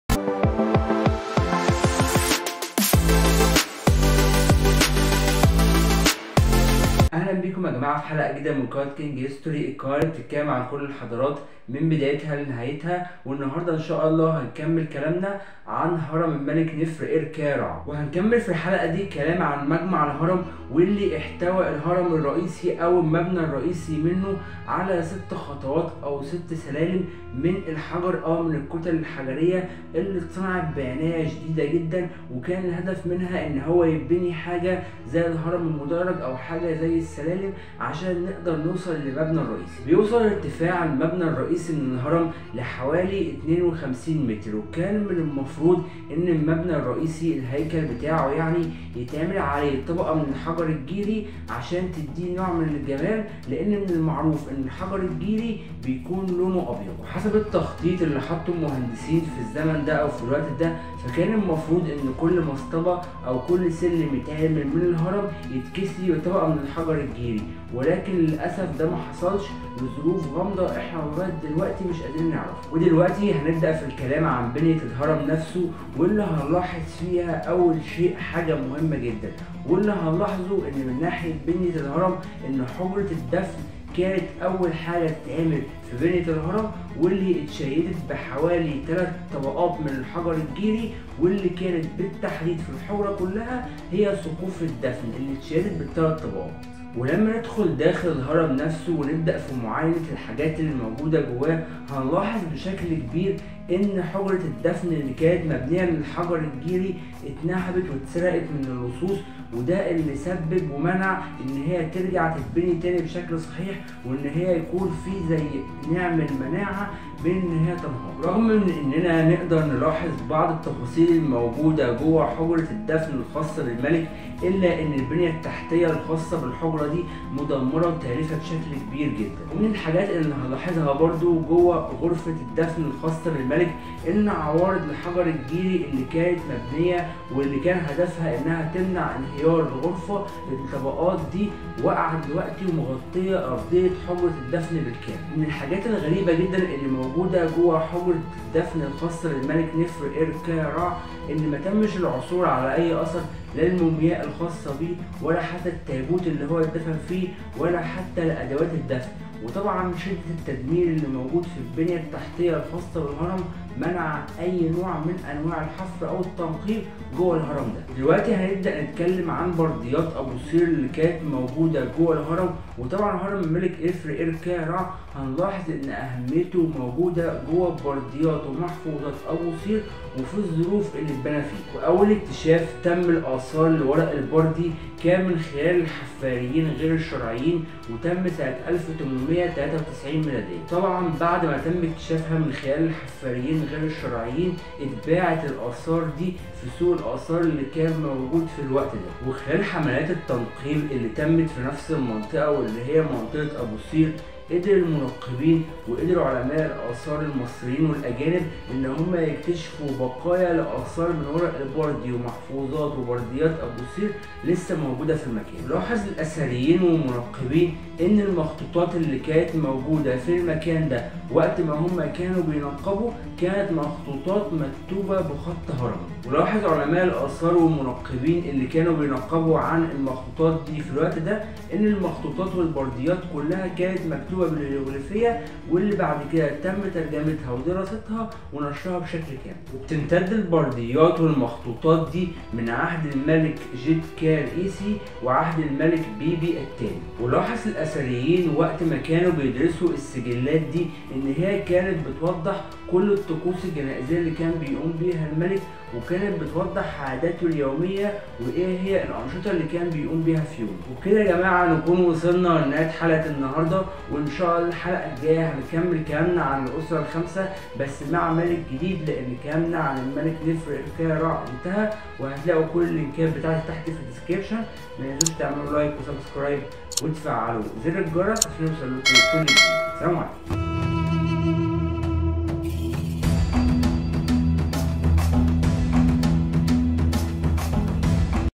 عصر الدولة القديمة (بناه الاهرام)_الاسره الخامسه_السر المخفى لهرم الملك نفر ار كارع|King History
النهارده هنكمل كلامنا عن الاسره الخامسه وبالتحديد عن هرم الملك نفر ار كارع وهنتكلم النهارده عن تصميم الهرم الرئيسى وعن بعض اجزاء المجمع الهرمى بالكامل وهنتكلم عن برديات ابوصير وعن اهميه الهرم التاريخية وهل فعلا الهرم اتسرق وهل دى كانت مجرد اكذوبه كل ده واكتر هتعرفوه فى الفيديو ان شاء الله